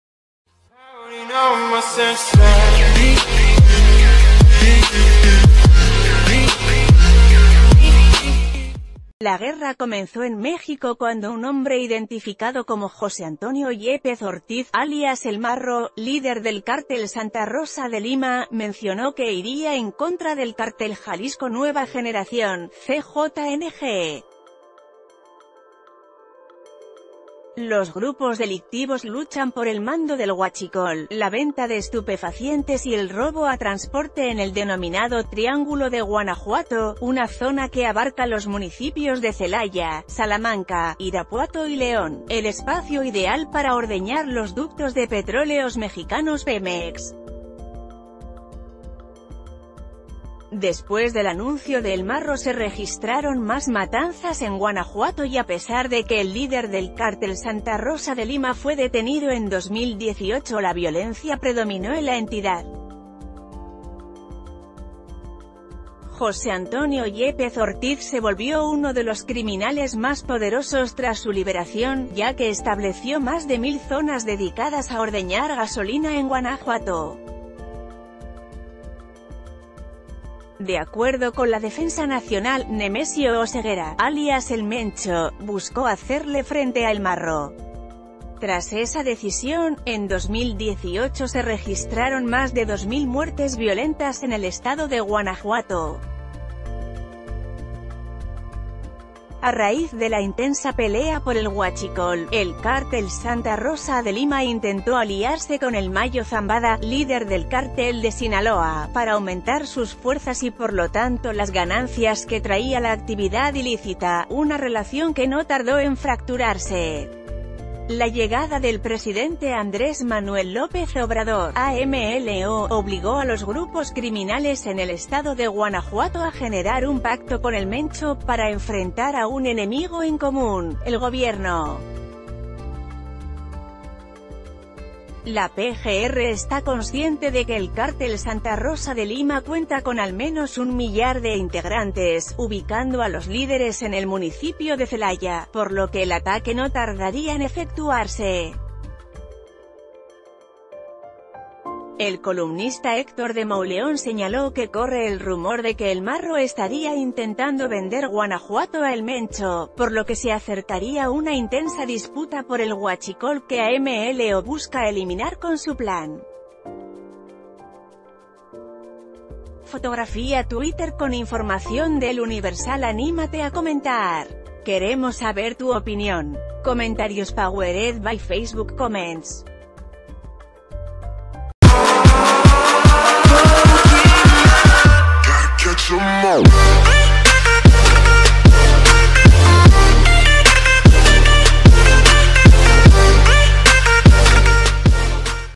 El Mencho y El Marro: la supuesta alianza contra el plan antihuachicol de AMLO
La guerra comenzó en México cuando un hombre identificado como José Antonio Yépez Ortiz, alias El Marro, líder del Cártel Santa Rosa de Lima, mencionó que iría en contra del Cártel Jalisco Nueva Generación (CJNG). Los grupos delictivos luchan por el mando del huachicol, la venta de estupefacientes y...